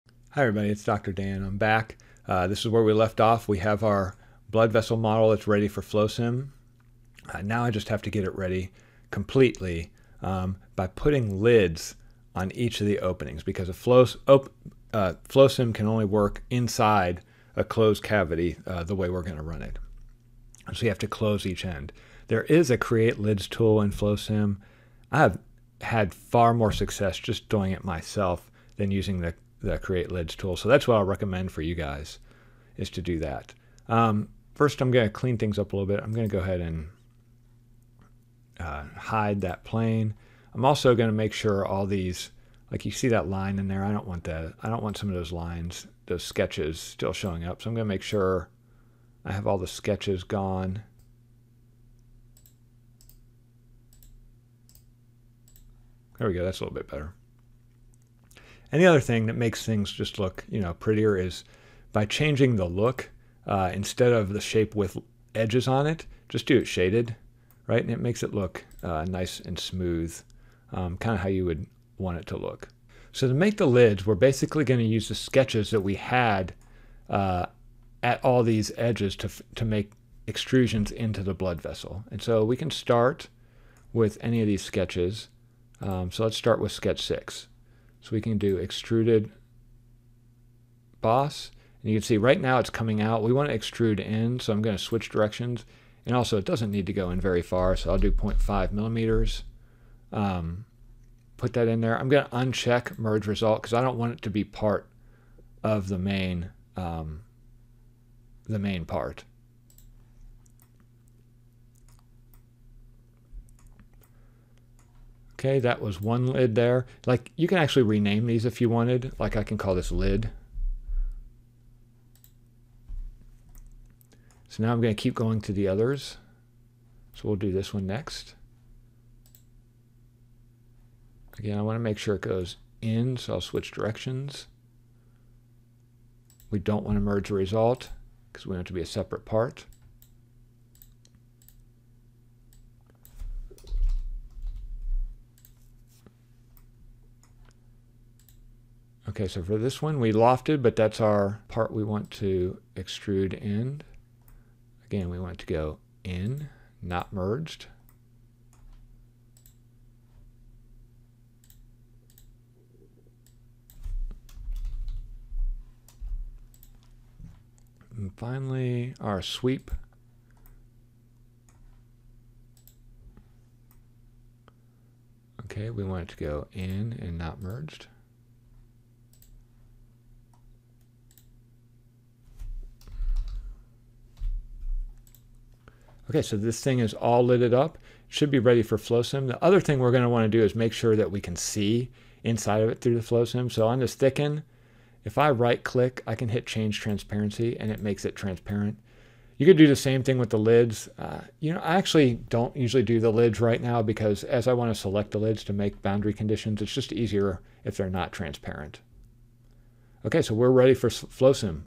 Modeling pipes / blood vessels in SOLIDWORKS - part 5
Fifth in a series of videos to model pipes or blood vessels in SOLIDWORKS using surfaces and converting to a solid part.  This video demonstrates adding caps to the model to prepare for the flow simulation